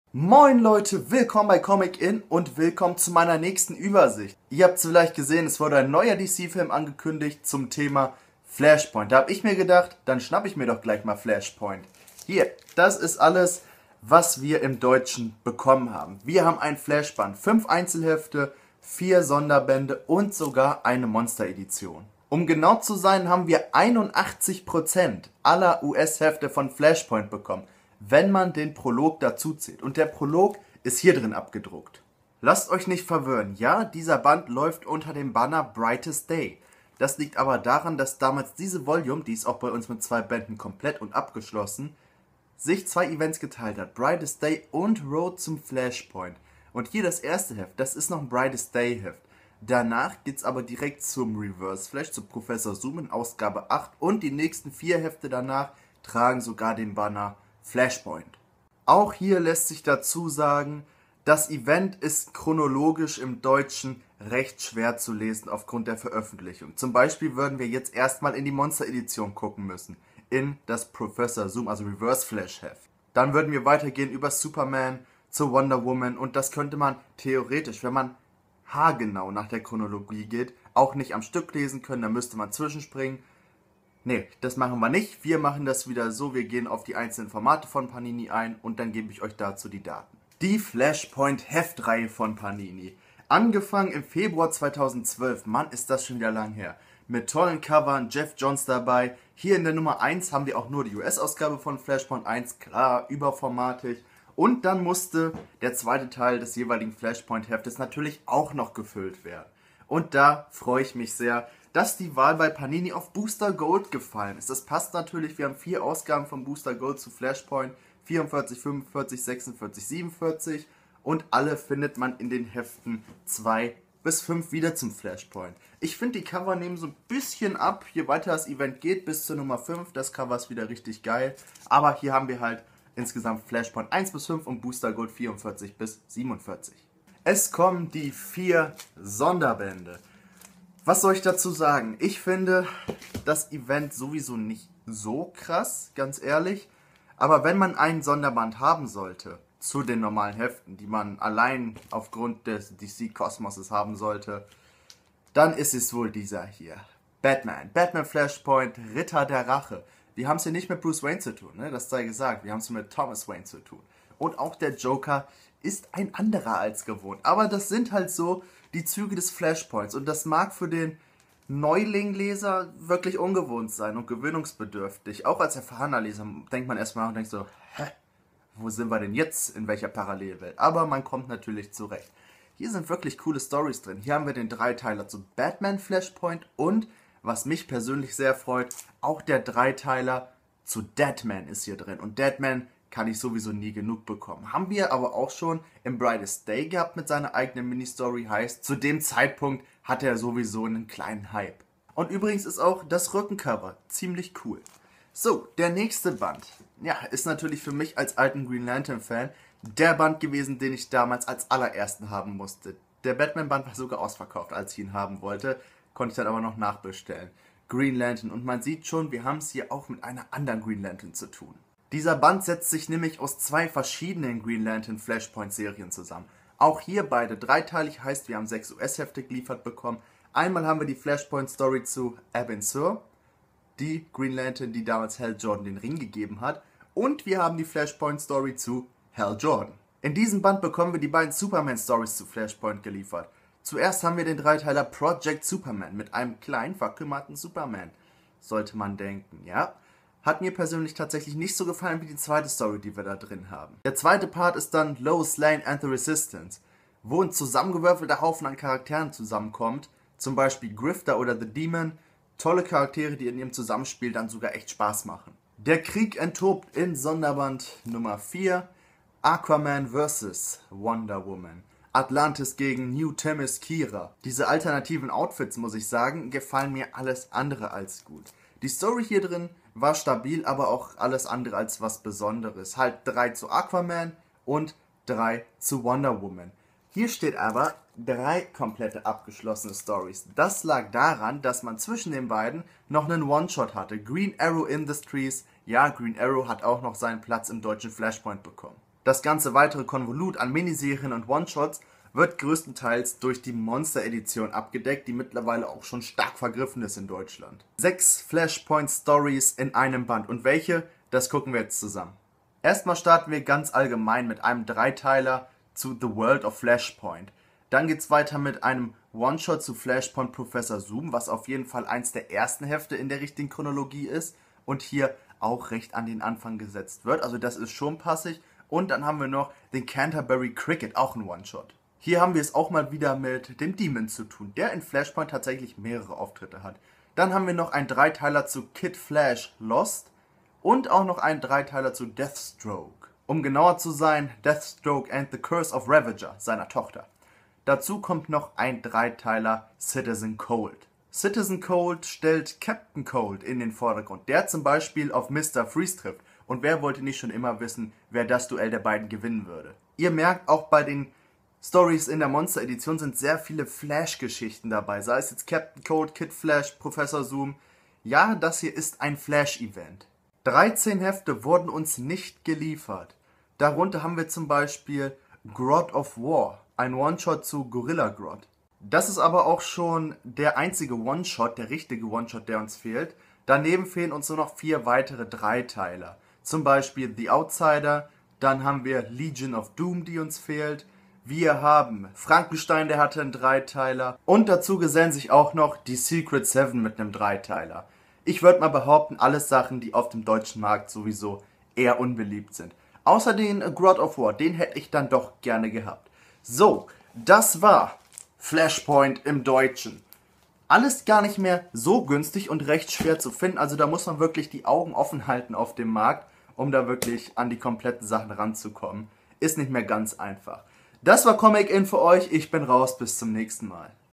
Der Flashpoint - Übersicht der deutschen Veröffentlichung! - Was haben wir bekommen/Was nicht?
Wir skippen erstmal Brightest Day (Mir fehlt 1 Tie-In Band) und beschäftigen uns mit dem Flashpoint!